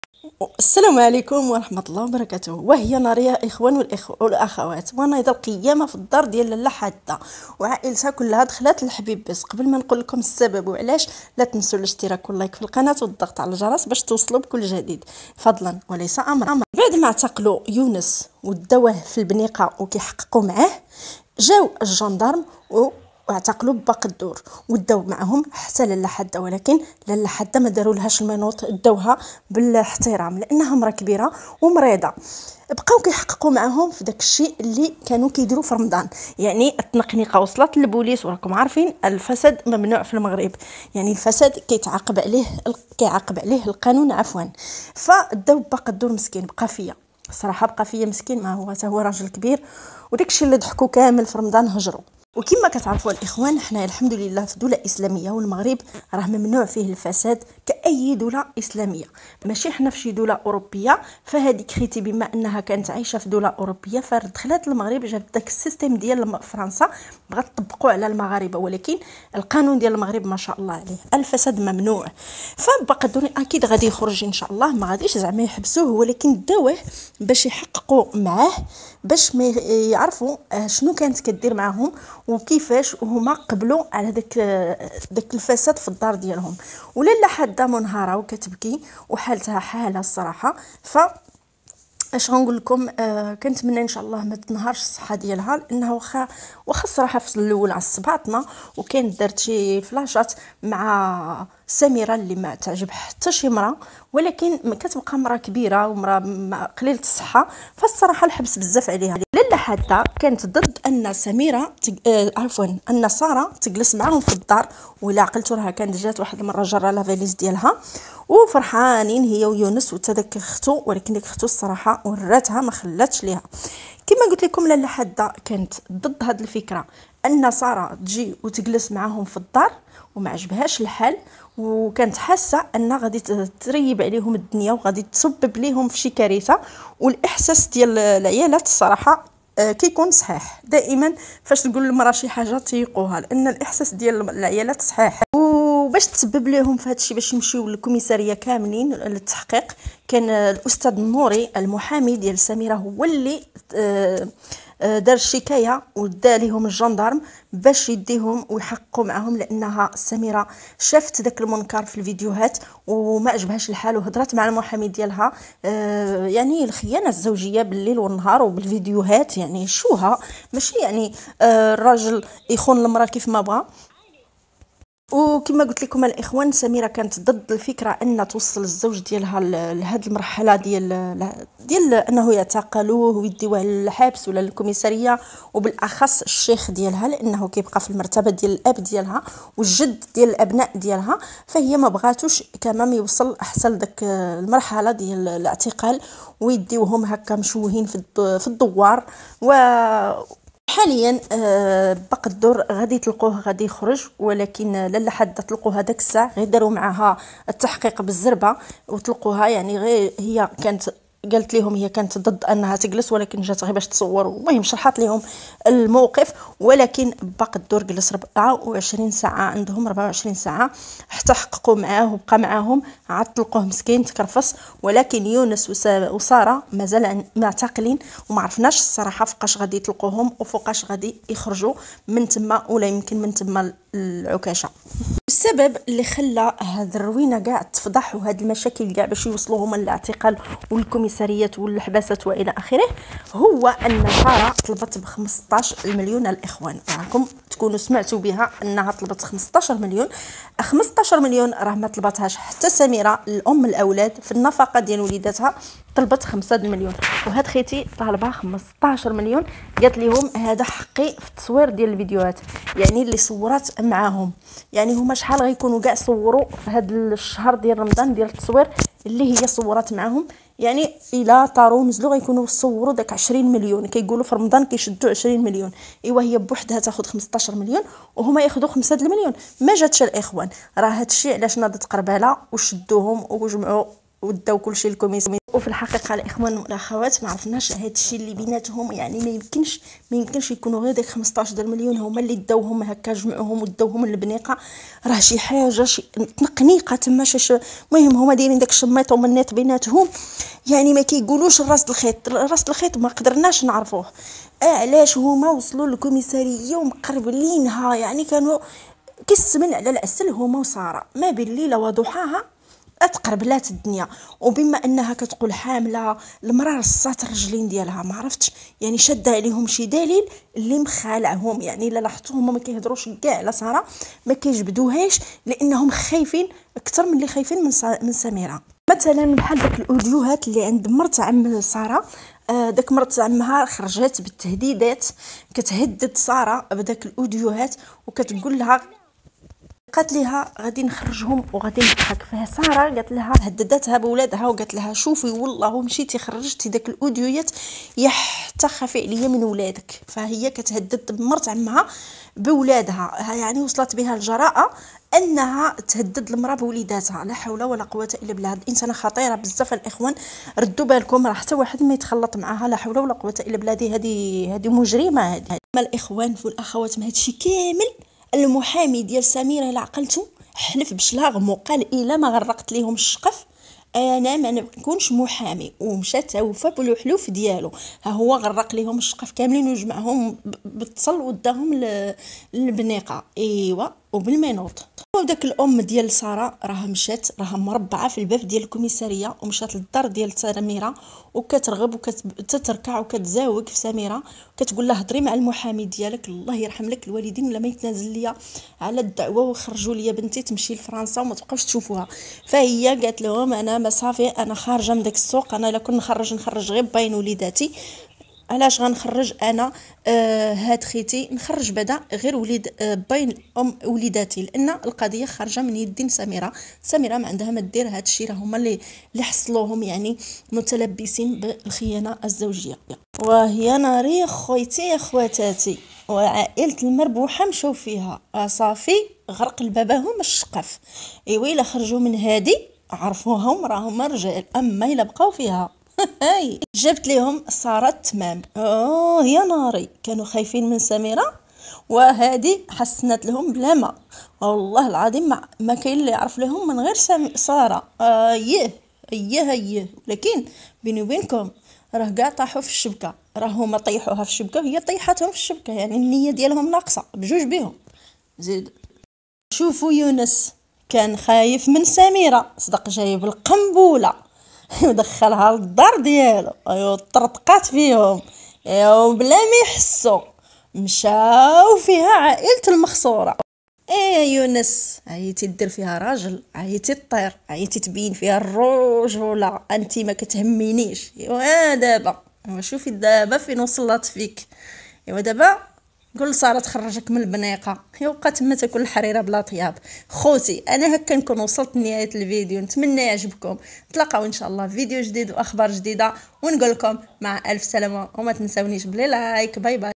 لحظات لا لاعتقال با قدور وسبب هي سارة ولاله حادة جاها انهيار عصبي نايضة قربالة
مي_نعيمة_البدوية_يونس_مربوحة# لاتنسو الاشتراك ولايك في القناه ليصلكم كل جديد